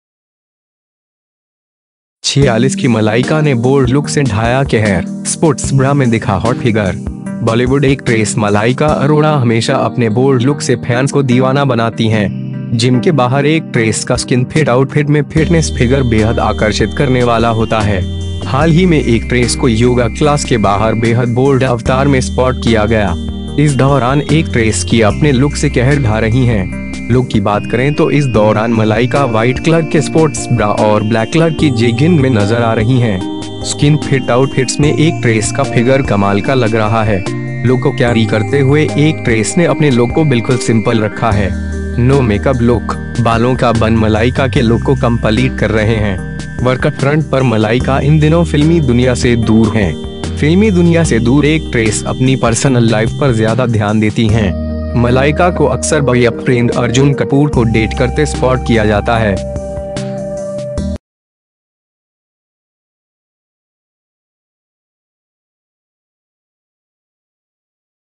46 की मलाइका ने बोल्ड लुक से ढाया कहर, स्पोर्टस ब्रा में दिखा हॉट फिगर
for mouth watering Indian Food Recipes 46 की मलाइका ने बोल्ड लुक से ढाया कहर, स्पोर्टस ब्रा में दिखा हॉट फिगर

बॉलीवुड एक्ट्रेस मलाइका अरोड़ा हमेशा अपने बोल्ड लुक से फैंस को दीवाना बनाती हैं। जिम के बाहर एक्ट्रेस का स्किनफिट आउटफिट में फिटनेस फिगर बेहद आकर्षित करने वाला होता है। हाल ही में एक्ट्रेस को योगा क्लास के बाहर बेहद बोल्ड अवतार में स्पॉट किया गया। इस दौरान एक्ट्रेस की अपने लुक से कहर ढा रही हैं।

लुक की बात करें तो इस दौरान मलाइका व्हाइट कलर के स्पोर्टस ब्रा और ब्लैक कलर की जेगिन्ग में नजर आ रही हैं।

स्किनफिट आउटफिट्स में एक्ट्रेस का फिगर कमाल का लग रहा है। लुक को कैरी करते हुए एक्ट्रेस ने अपने लुक को बिल्कुल सिंपल रखा है।

नो मेकअप लुक, बालों का बन मलाइका के लुक को कम्पलीट कर रहे हैं।

वर्कफ्रंट पर मलाइका इन दिनों फिल्मी दुनिया से दूर हैं। फिल्मी दुनिया से दूर एक्ट्रेस अपनी परसनल लाइफ पर ज्यादा ध्यान देती हैं।

मलाइका को अक्सर बॉयफ्रेंड अर्जुन कपूर को डेट करते स्पॉट किया जाता है।